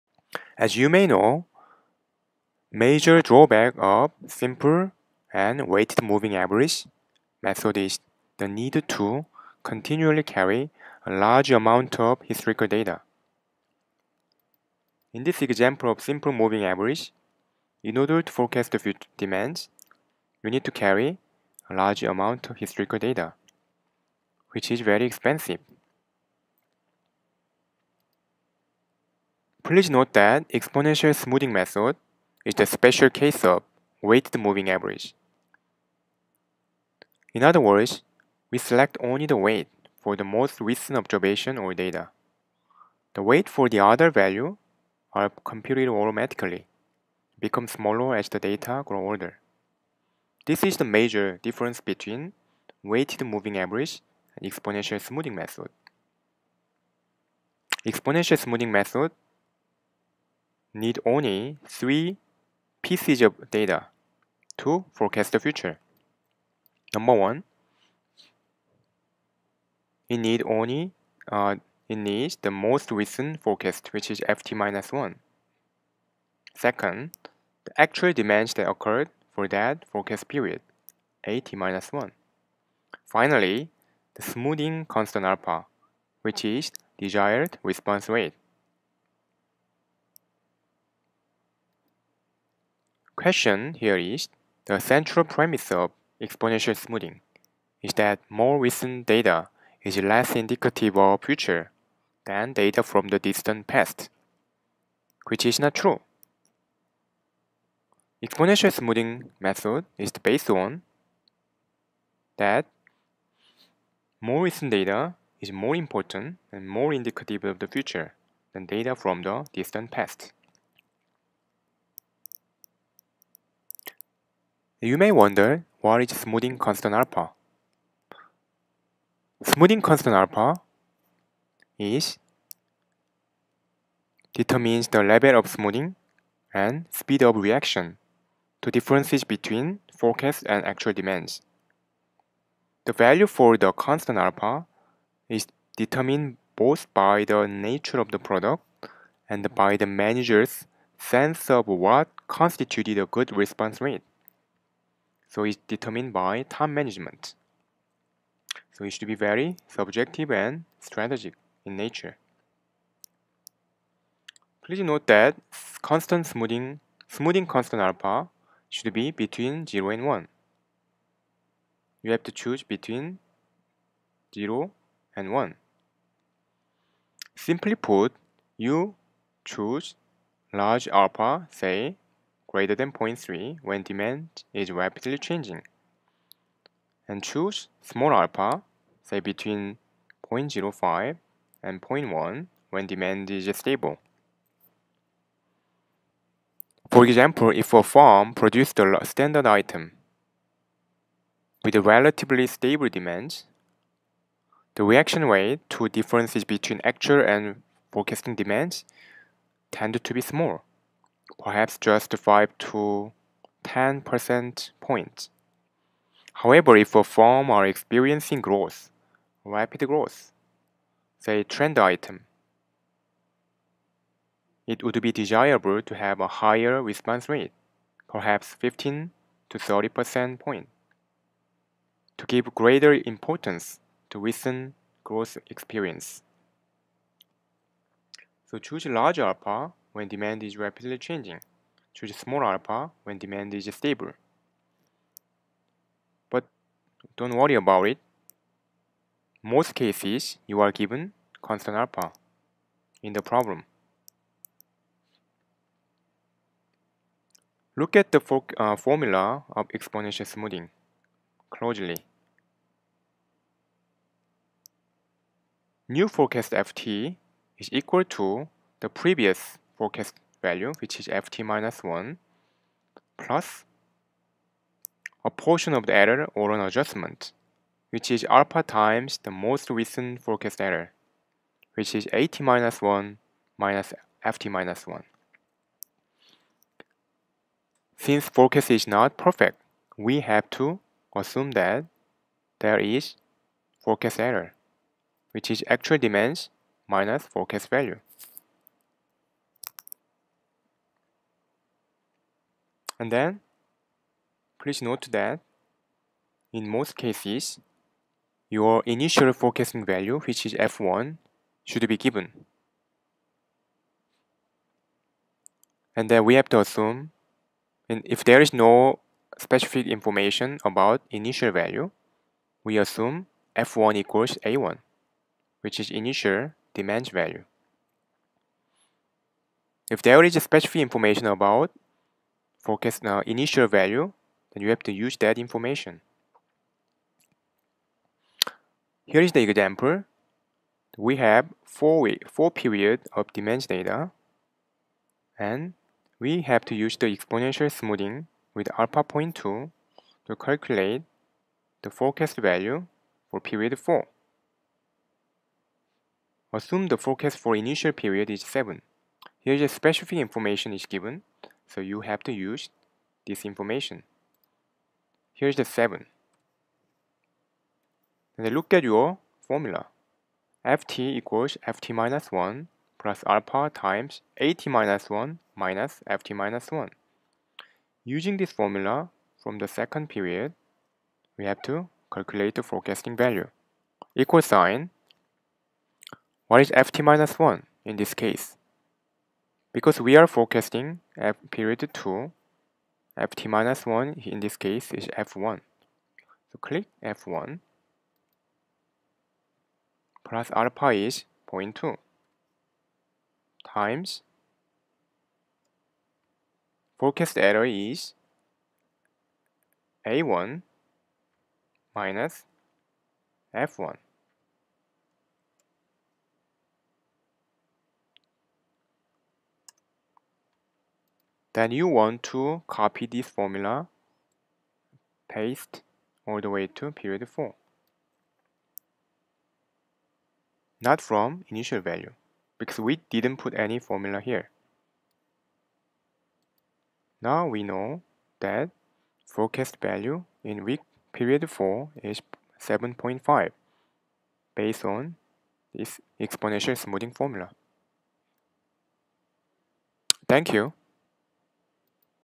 Lecture 3-3 Exponential Smoothing
This video explains the time series method (Exponential Smoothing).